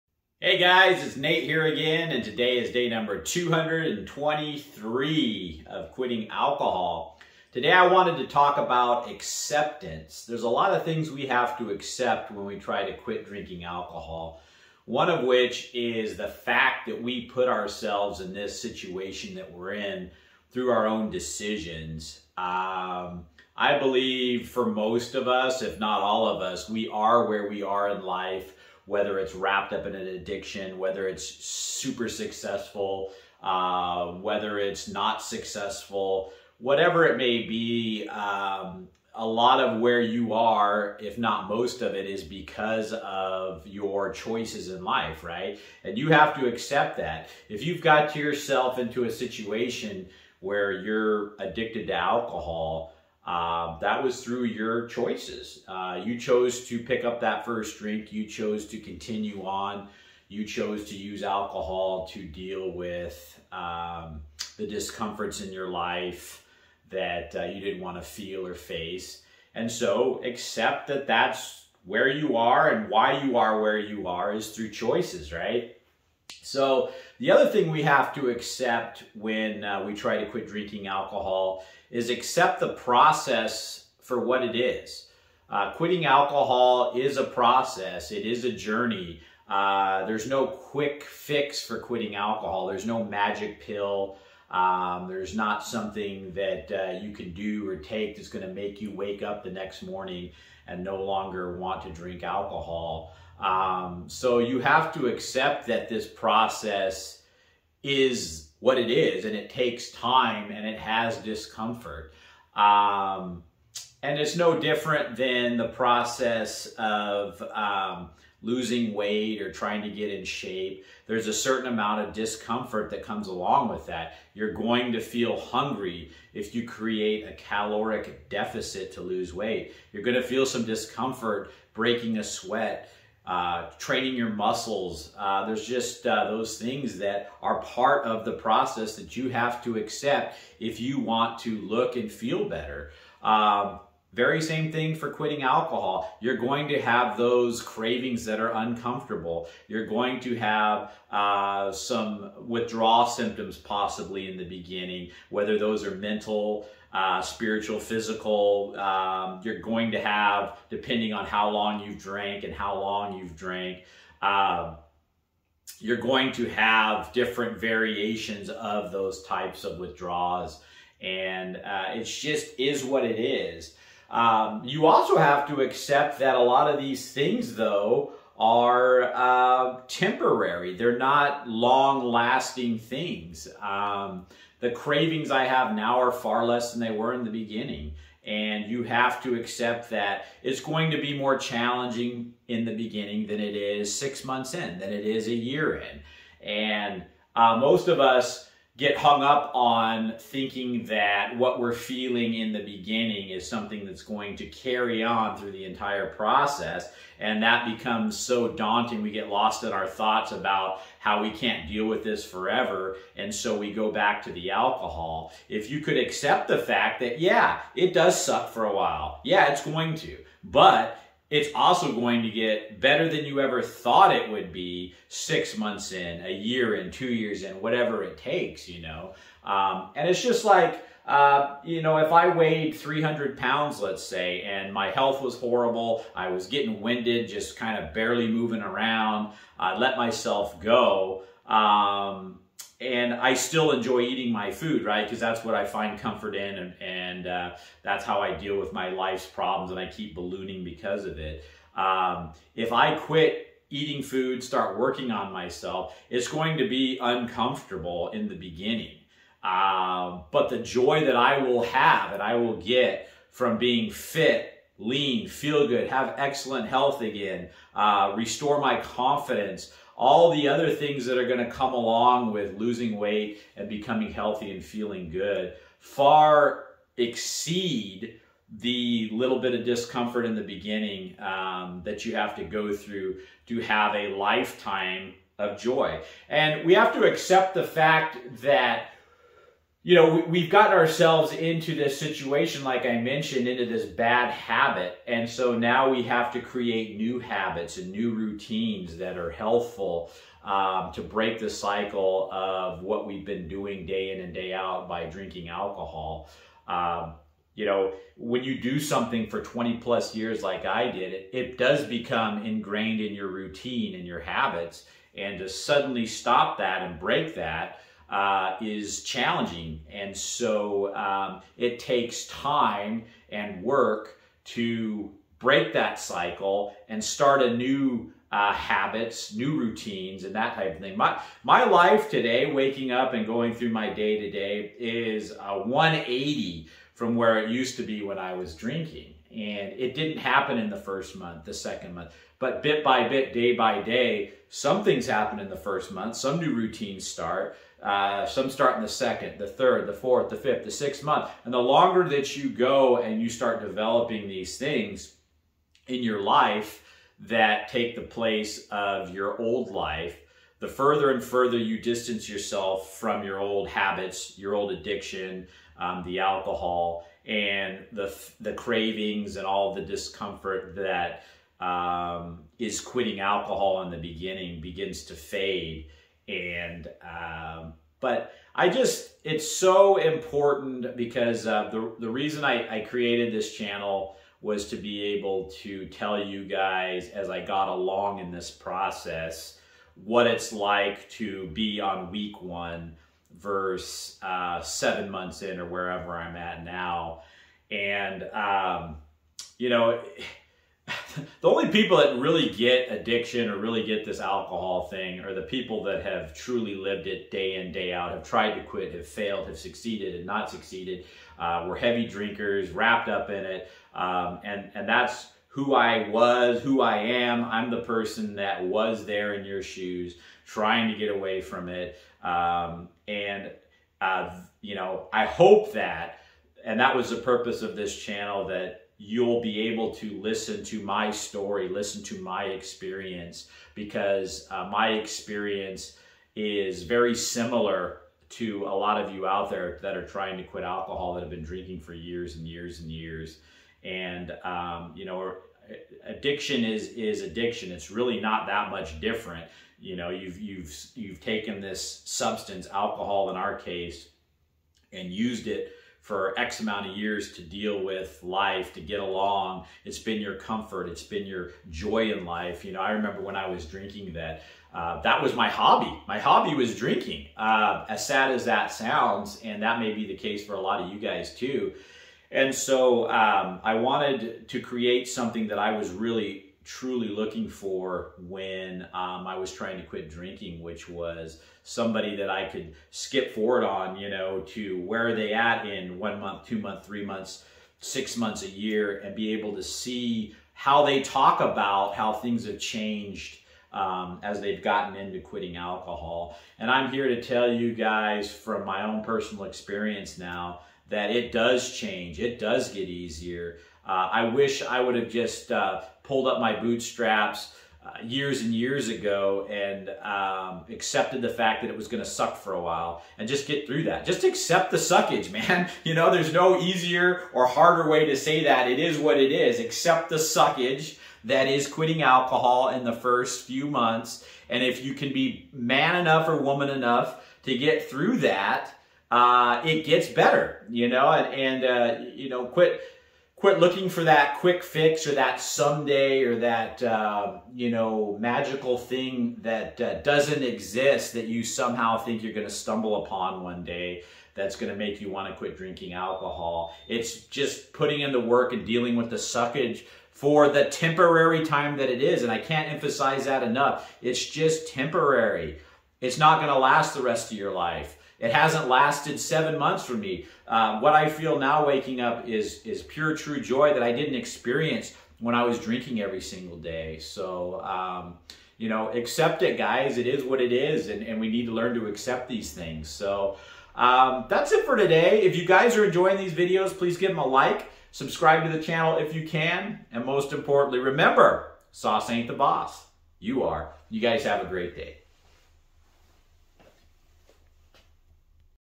Learning To Accept The Process of Quitting Alcohol (Day 223 No Alcohol)
In this video I talk about how there is no "quick fix" to quitting alcohol and how you just have to embrace and accept the process for what it is.